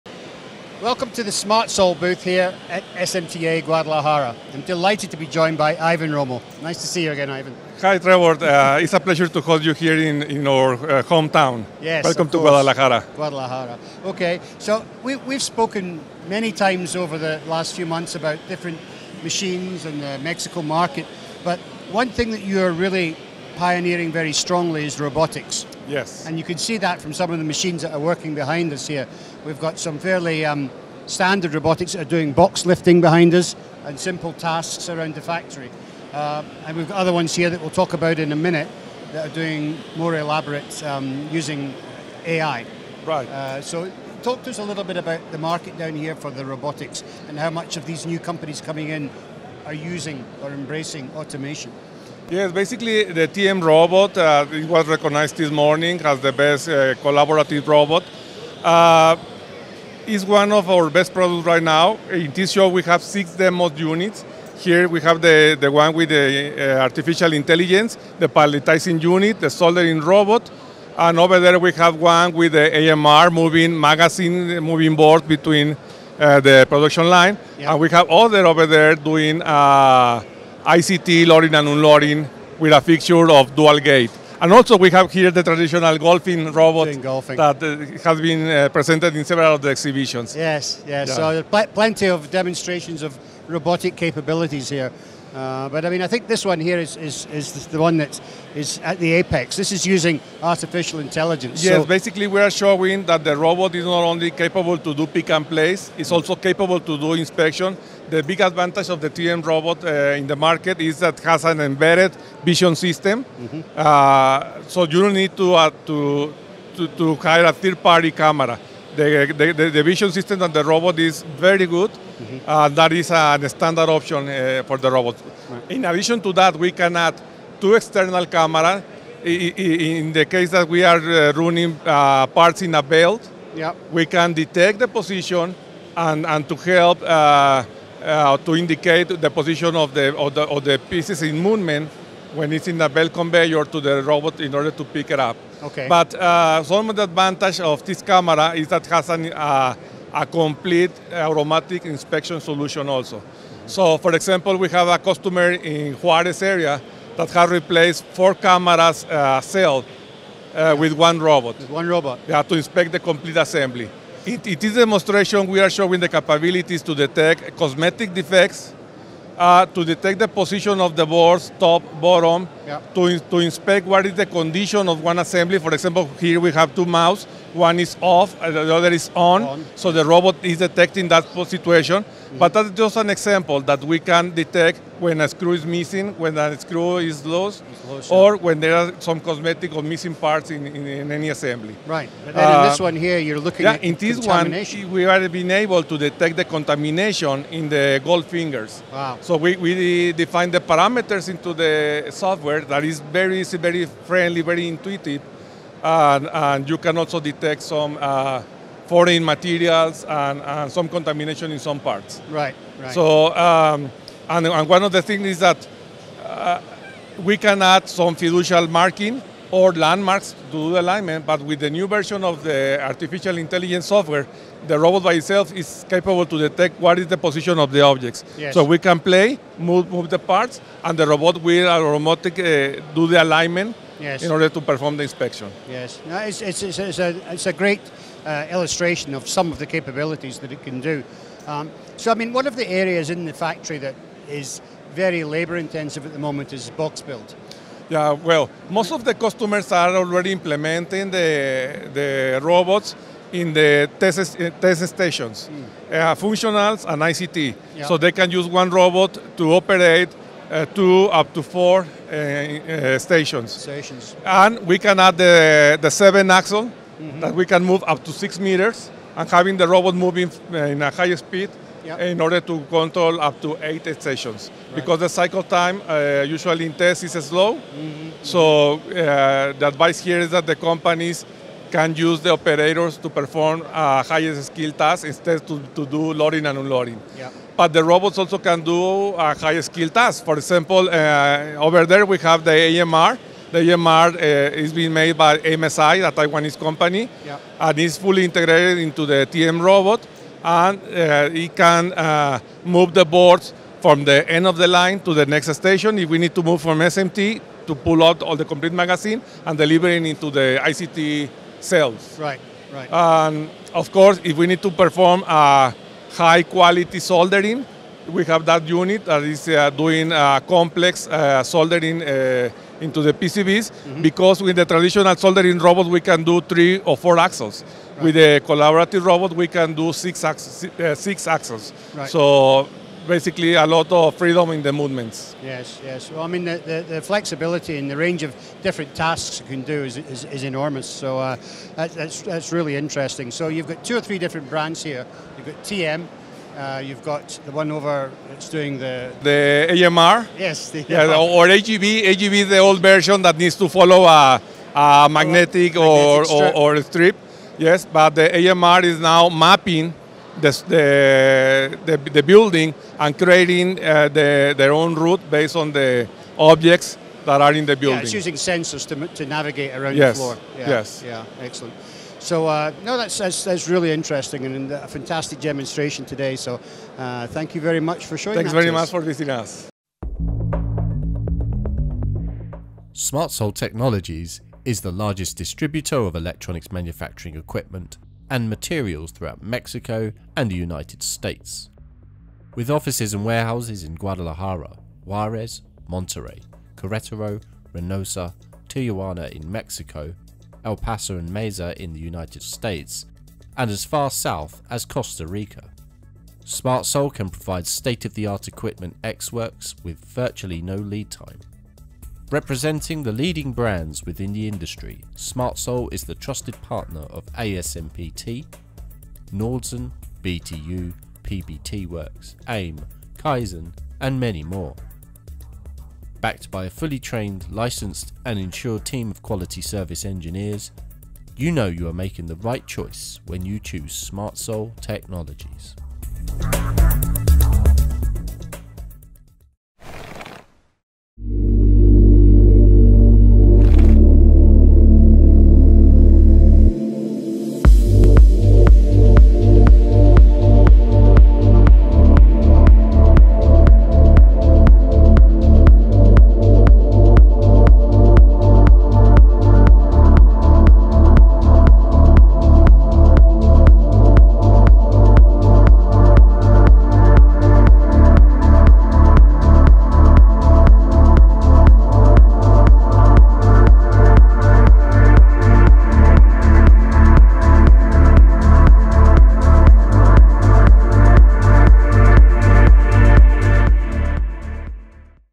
SmartSol display their robotic automation systems
SmartSol represent a wide range of robots and automation systems for a variety of applications, ranging from simple box shifting to mafazine loading/unloading, soldering robots and intelligent, AI driven systems.

00:00 - Intro /  Talk to us about the market in Mexico for robotics
01:31 - This robot is using artificial intelligence...
04:01 - Box build is an area in the factory which is labor intensive!
05:37 - You have some different brands here!